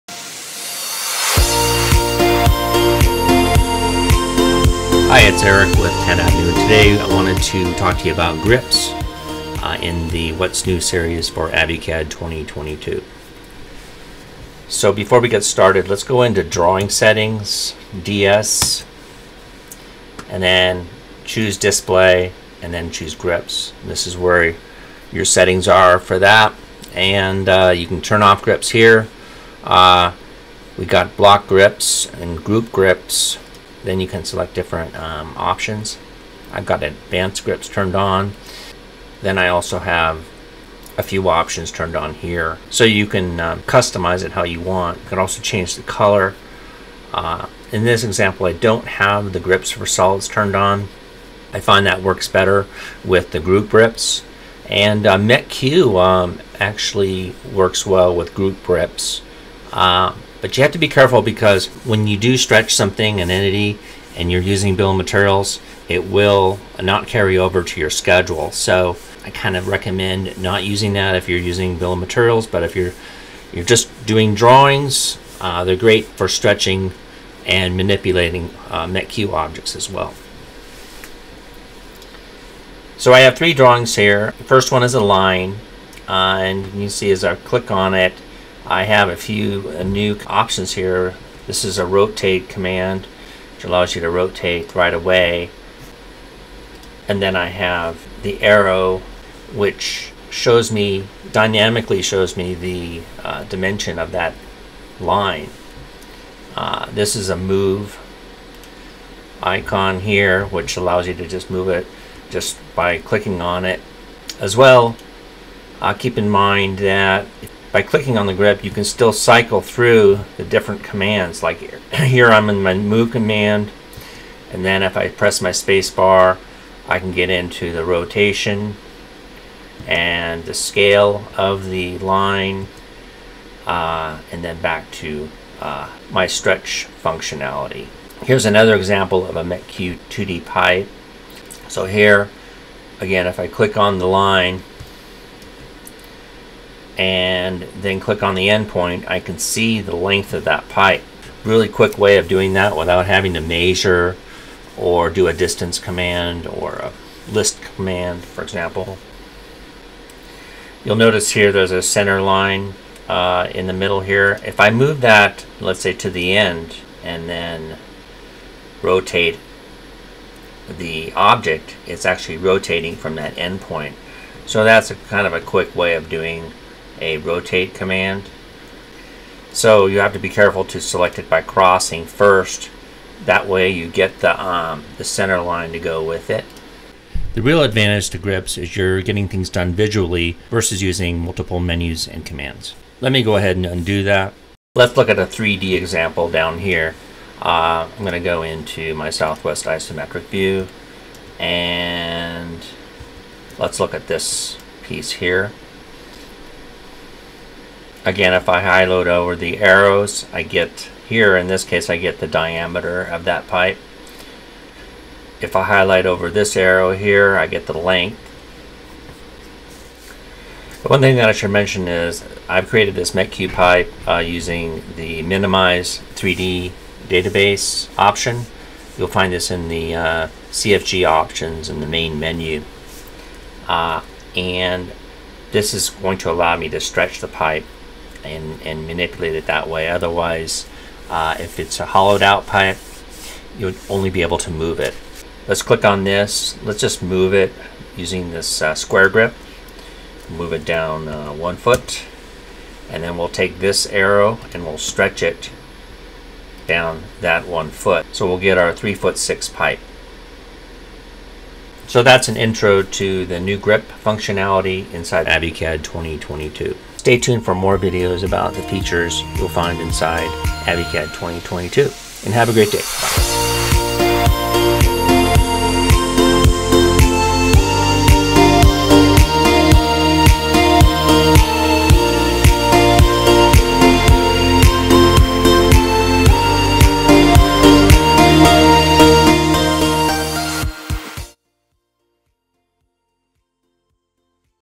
Advanced Grips in CAD - for AViCAD 2022
How to use Advanced Grips in AViCAD (an Acad alternative)